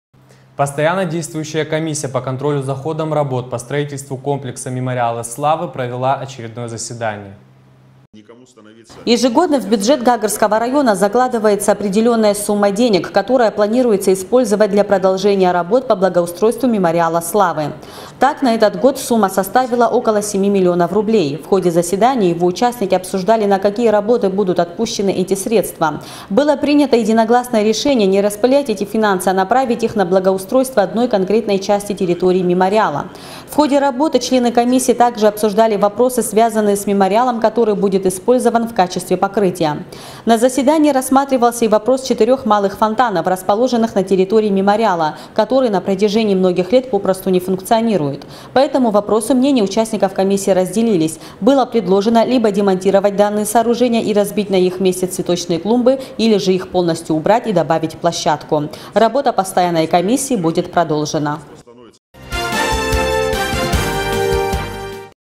Заседание комиссии по строительству Мемориала Славы
Постоянно действующая комиссия по контролю за ходом работ по строительству комплекса Мемориала Славы провела очередное заседание.